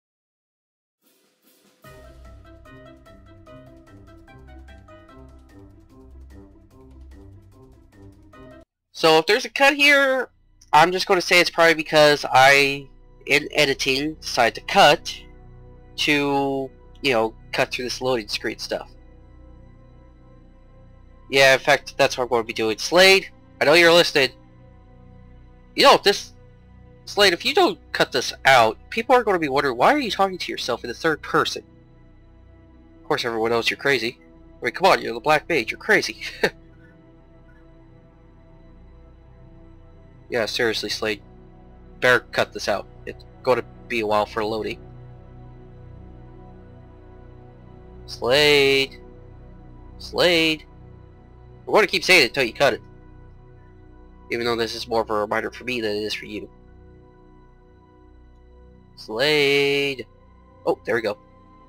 Slade talks to Slade | Deleted Scene
A deleted scene from Game 2's first video of beyond earth that I was going to throw away but decided to share. I've gone crazy.